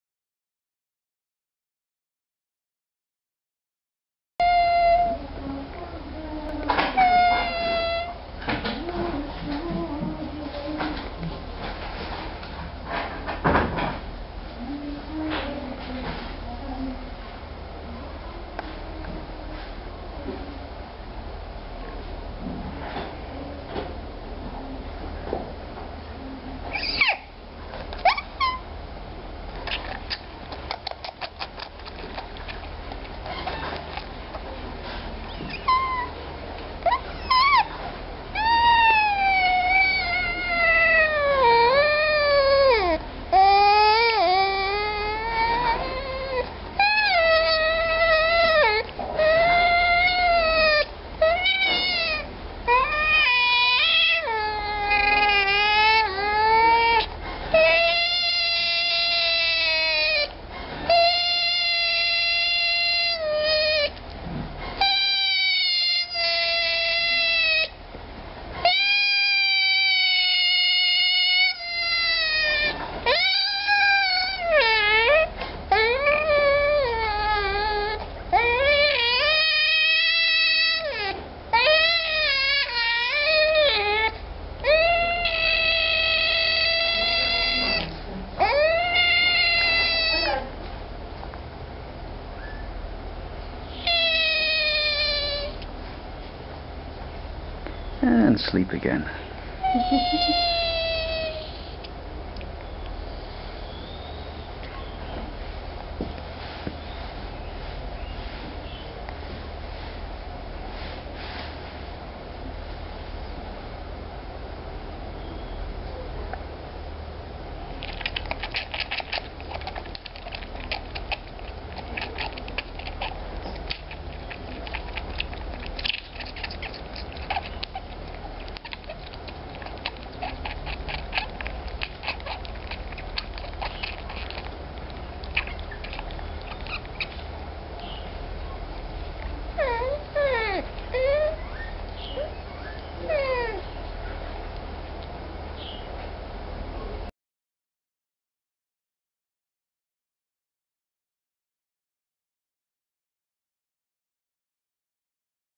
Otter's got talent
Adorable rare baby African Clawless Otter cant decide whether to eat, sleep or sing